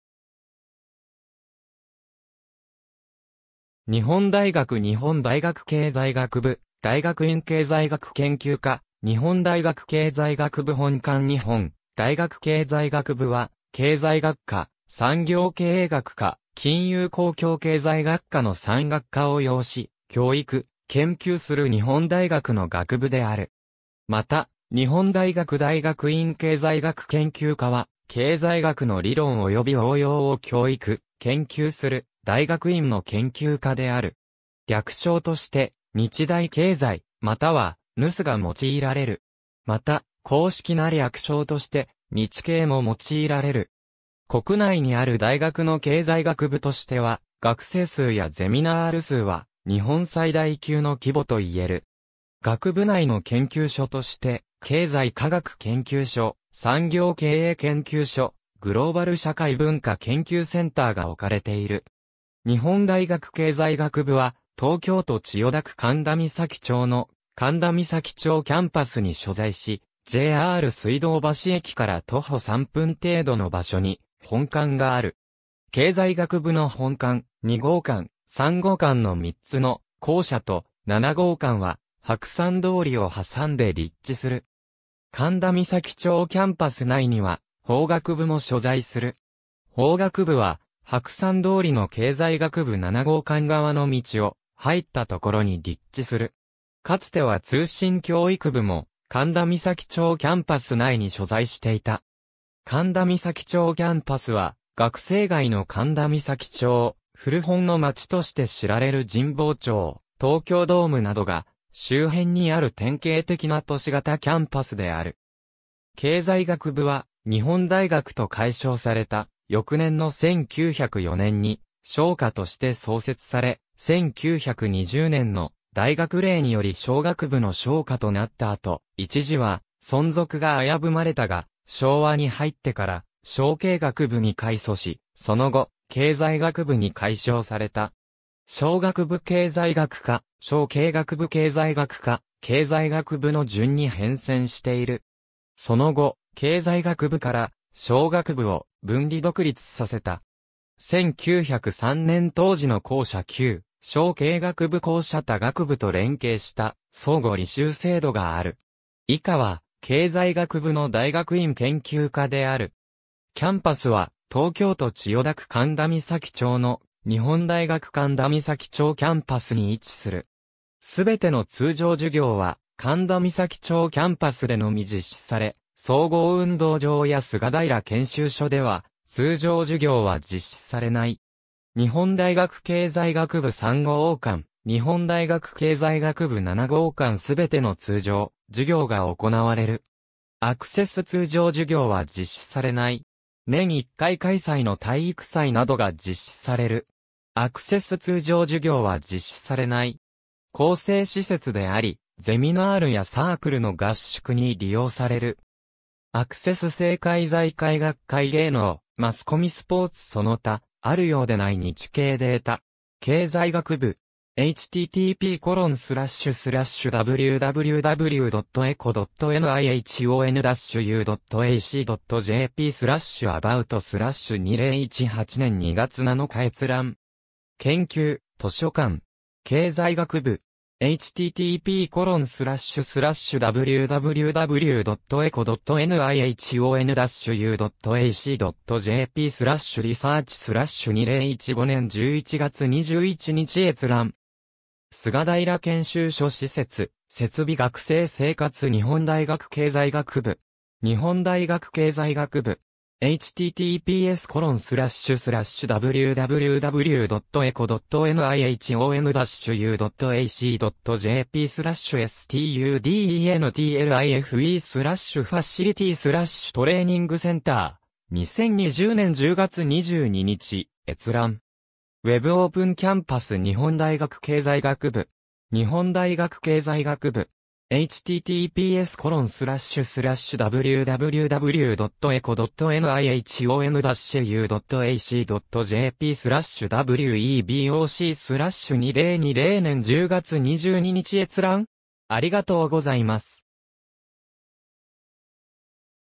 日本大学経済学部・大学院経済学研究科
日本大学 」 日本大学経済学部・大学院経済学研究科 日本大学経済学部本館 日本大学経済学部（にほんだいがくけいざいがくぶ、Nihon University College of Economics）は、経済学科・産業経営学科・金融公共経済学科の3学科を擁し、教育・研究する日本大学の学部である。
また 、日本大学大学院経済学研究科（にほんだいがくだいがくいんけいざいがくけんきゅうか）は、経済学の理論および応用を教育・研究する大学院の研究科である。
略称として、日大経済（にちだいけいざい）またはNUCEが用いられる。
また、公式な略称として、ニチケイ（Nichikei）も用いられる。
国内にある大学の経済学部としては、学生数やゼミナール数は日本最大級の規模といえる。
学部内の研究所として、経済科学研究所・産業経営研究所・グローバル社会文化研究センターが置かれている。
日本大学経済学部は、東京都千代田区神田三崎町の神田三崎町キャンパスに所在し、JR水道橋駅から徒歩3分程度の場所に本館がある。
経済学部の本館・2号館・3号館の3つの校舎と、7号館は、白山通りを挟んで立地する。
神田三崎町キャンパス内には、法学部も所在する。
法学部は、白山通りの経済学部7号館側の道を入ったところに立地する。
かつては通信教育部も、神田三崎町キャンパス内に所在していた。
神田三崎町キャンパスは、学生街の神田三崎町、古本の町として知られる神保町、東京ドームなどが周辺にある典型的な都市型キャンパスである。
経済学部は、日本大学と改称された翌年の1904年に「商科」（Department of Commerce）として創設され、1920年の大学令により商学部（College of Commerce）の商科（Department of Commerce）となったあと、一時は存続が危ぶまれたが、昭和に入ってから「商経学部」（College of Commerce and Economics）に改組し、その後、「経済学部」に改称された。
商学部経済学科、商経学部経済学科、経済学部の順に変遷している。
その後、経済学部から商学部を分離独立させた。
1903年（明治36年）当時の校舎 旧・商経学部校舎（1938年頃） 他学部と連携した相互履修制度がある。
以下は、経済学部の大学院研究科(博士前期課程・博士後期課程)である。
キャンパスは、東京都千代田区神田三崎町の日本大学神田三崎町キャンパスに位置する。
すべての通常授業は、神田三崎町キャンパスでのみ実施され、総合運動場(上福岡グラウンド)や菅平研修所では通常授業は実施されない。
日本大学経済学部3号館 日本大学経済学部7号館（手前のビル。奥は東京歯科大学水道橋校舎新館） すべての通常授業が行われる。
アクセス 通常授業は実施されない。
年1回開催の体育祭などが実施される。
厚生施設であり、ゼミナールやサークルの合宿に利用される。
アクセス 政界 財界 学界 芸能・マスコミ スポーツ その他 ^ “あるようでない ニチケイデータ”. 経済学部（ニチケイ）. 2018年2月7日閲覧。
^ “研究・図書館”. 経済学部（ニチケイ）. 2015年11月21日閲覧。
^ “菅平研修所｜施設・設備｜学生生活｜日本大学経済学部”. 日本大学経済学部. (2020年9月30日). 2020年10月22日閲覧。
^ “Web Open Campus｜日本大学経済学部”. 日本大学経済学部. (2020年10月4日). 2020年10月22日閲覧。